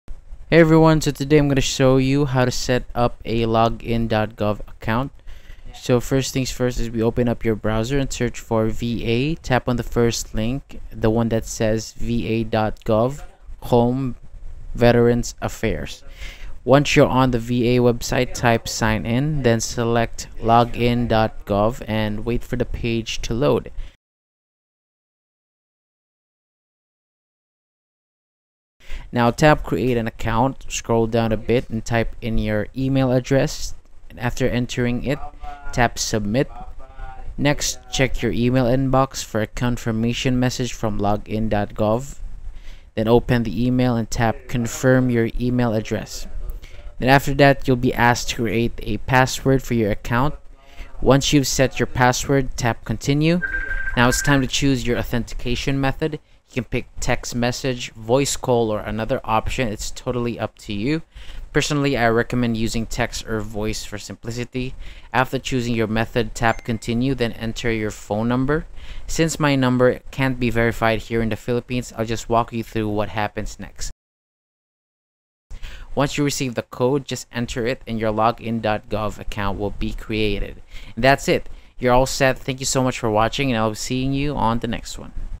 HOW TO SETUP A LOGIN.GOV ACCOUNT (FULL GUIDE 2026)
🔐 How to Set Up a Login.gov Account | Full Step-by-Step Guide
Need access to U.S. government services online? In this tutorial, I’ll walk you through how to set up a Login.gov account—your secure gateway to services like USAJOBS, TSA PreCheck, Social Security, FEMA, and more.

Creating a Login.gov account is fast, free, and essential for accessing many official federal websites.

💡 In this video, you'll learn:

How to sign up for a Login.gov account

How to verify your email and create a secure password

How to set up two-factor authentication (2FA)

What to do if you're locked out or forgot your password

Tips for keeping your account safe and secure

✅ Works for U.S. citizens and eligible residents
✅ Required for many government programs and applications
✅ Mobile and desktop-friendly process

⚠️ Important: Always use accurate personal information and keep your 2FA device or backup codes safe.